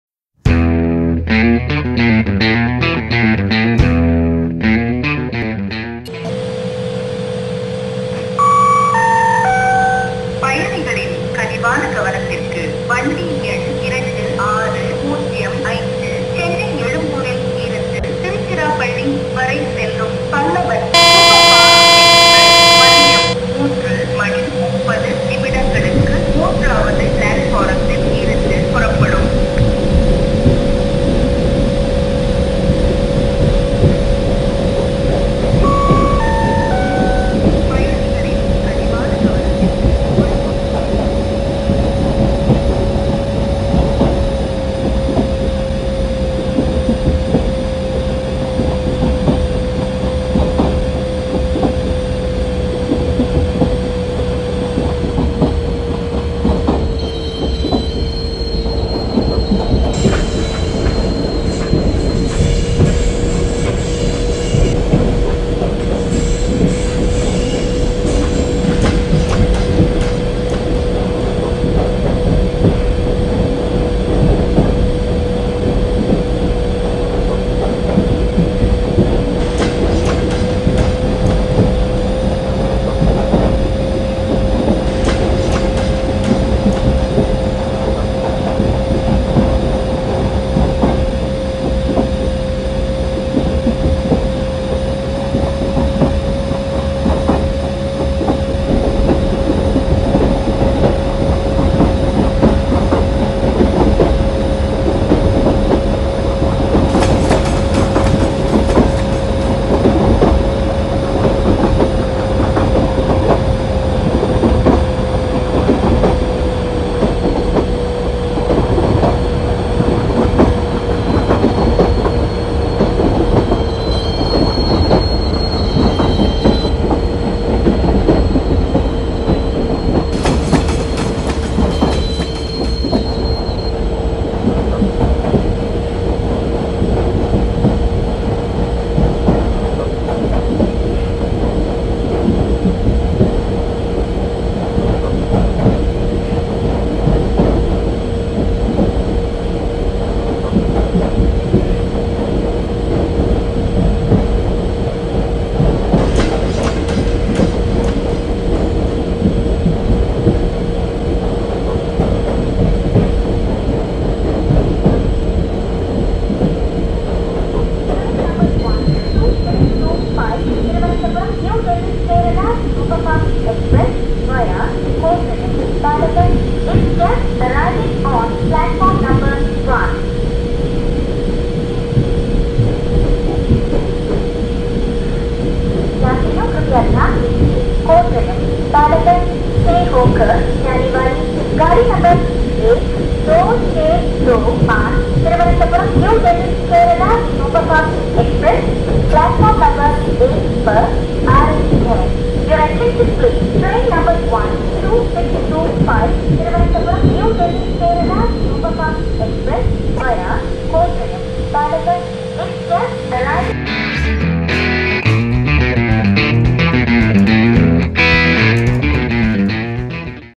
Cockpit view Chennai to Arakkonam Local Train Sim
Watch most exciting cockpit view of Chennai to Arakkonam suburban train - Train Sim